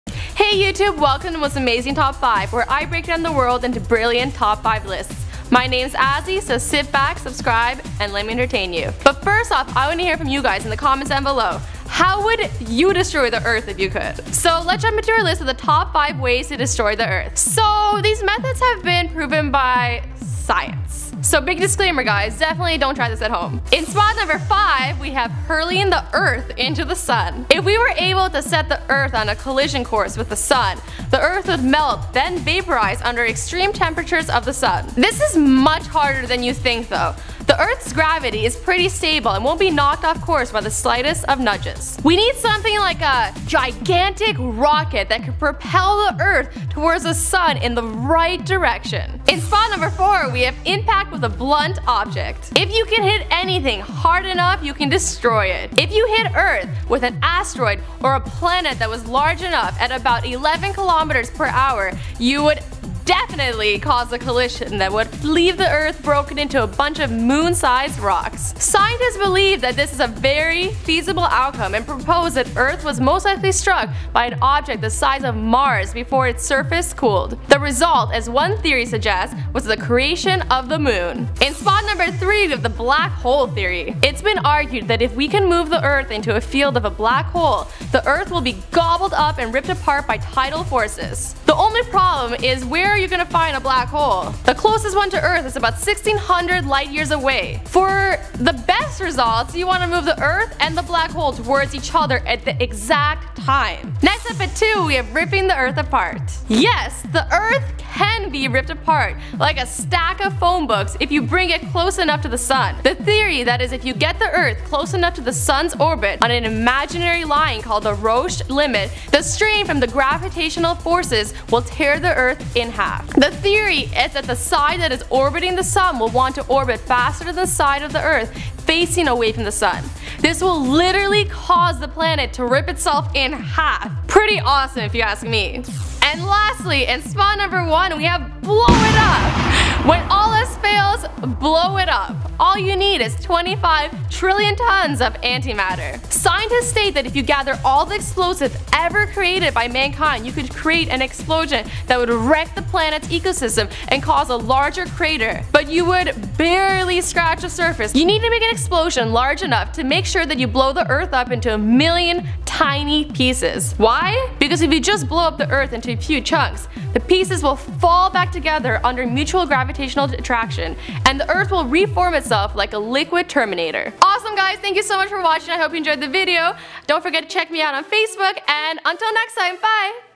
Top 5 Ways to Destroy The Earth
There are many theories of how we can destroy earth.  So we are giving you the Top 5 Ways To Destroy The Earth.